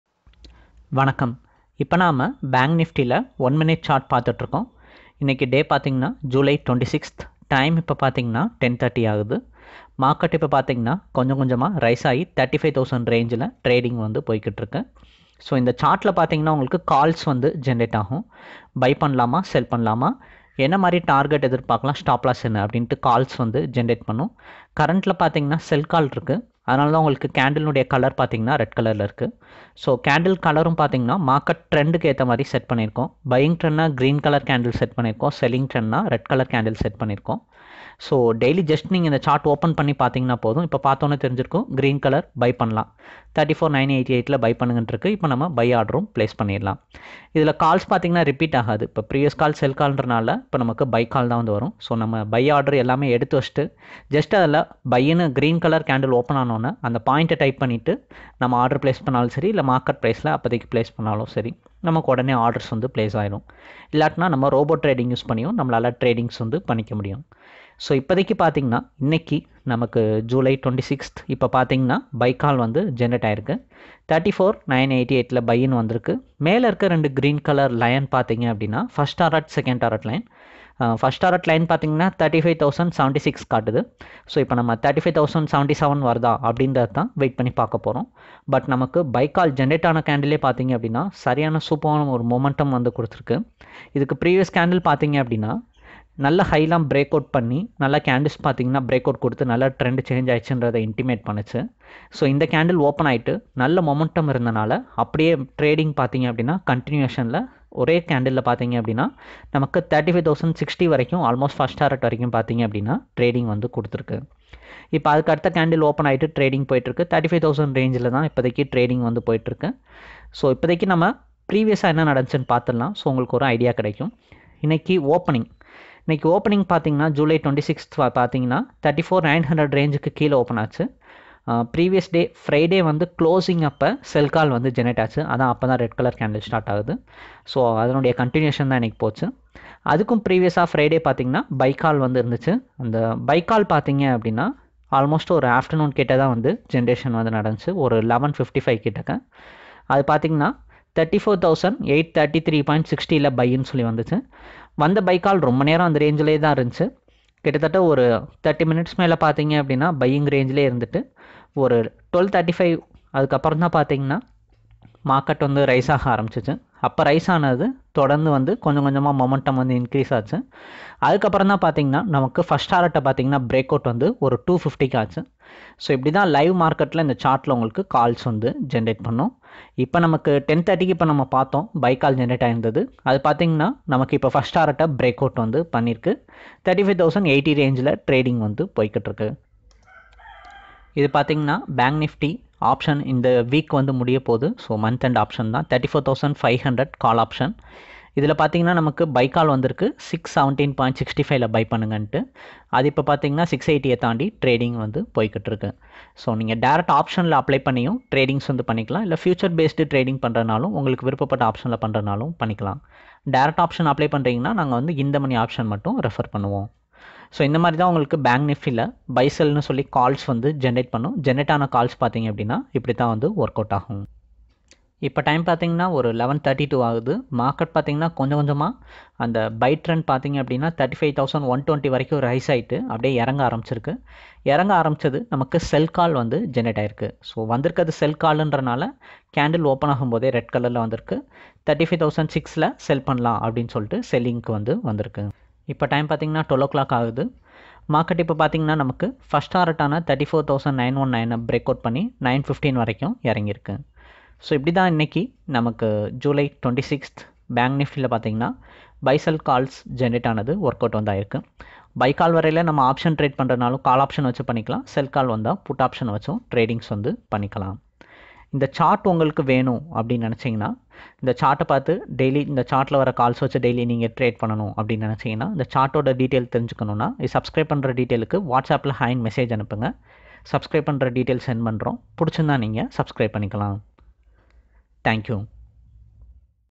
Bank Nifty Option Trading Using Scalping Technique in 1 Minute Chart | Live Intraday Trading
Bank Nifty Option Scalping Trading Techniques analysed in 1 Minute Chart, Live Market Intraday Trading Analysis.

⏱️⏱️TIME CODES⏱️⏱️
0:00​ - Intro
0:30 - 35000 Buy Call
1:00 - Calls Details
2:00​ - Market trend analysis
3:00​ - In the money option